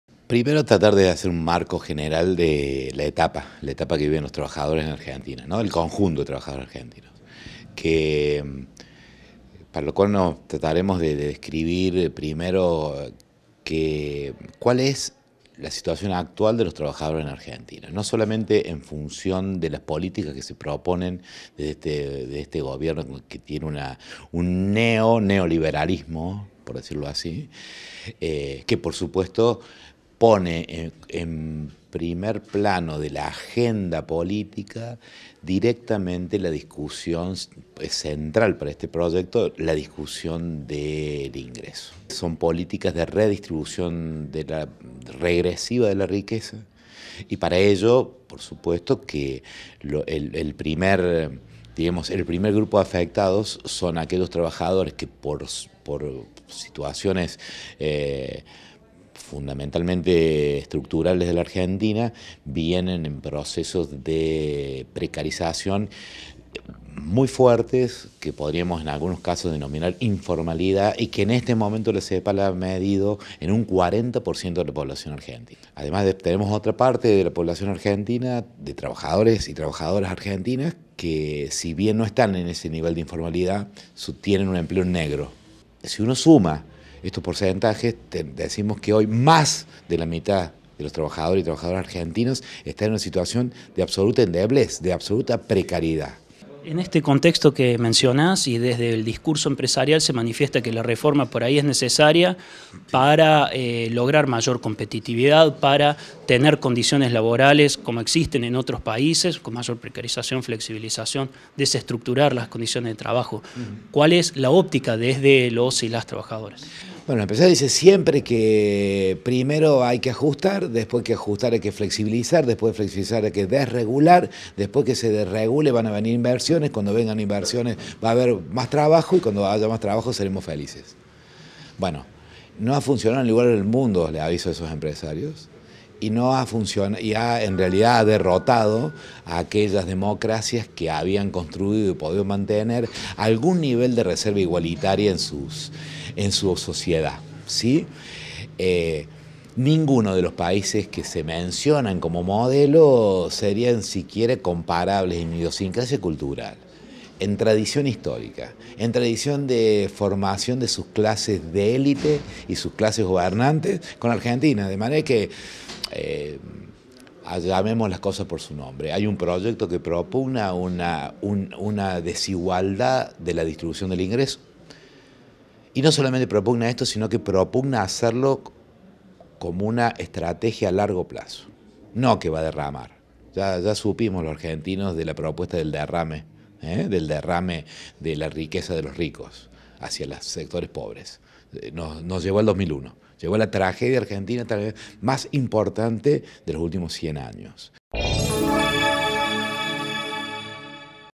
PLAN DE LUCHA DOCENTE - EUGENIO BIAFORE
REPASARON PUNTOS DE LA REFORMA LABORAL IMPULSADA POR EL GOBIERNO
Facebook.com/uniriotv
Twitter.com/uniriotv